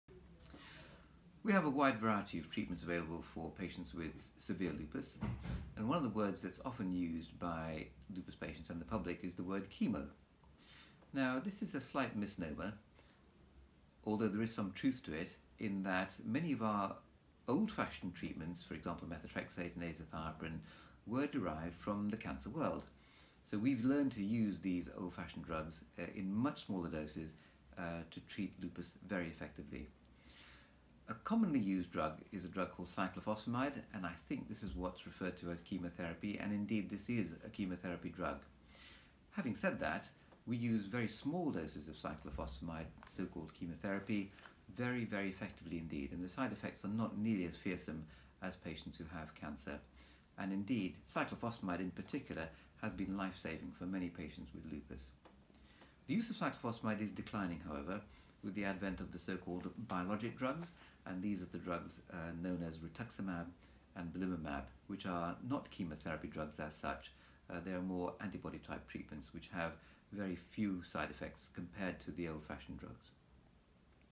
Professor David D'Cruz talks about chemotherapy and lupus
The word chemo is sometimes used to describe treatments for lupus. Professor David D'Cruz clarifies exactly what this means.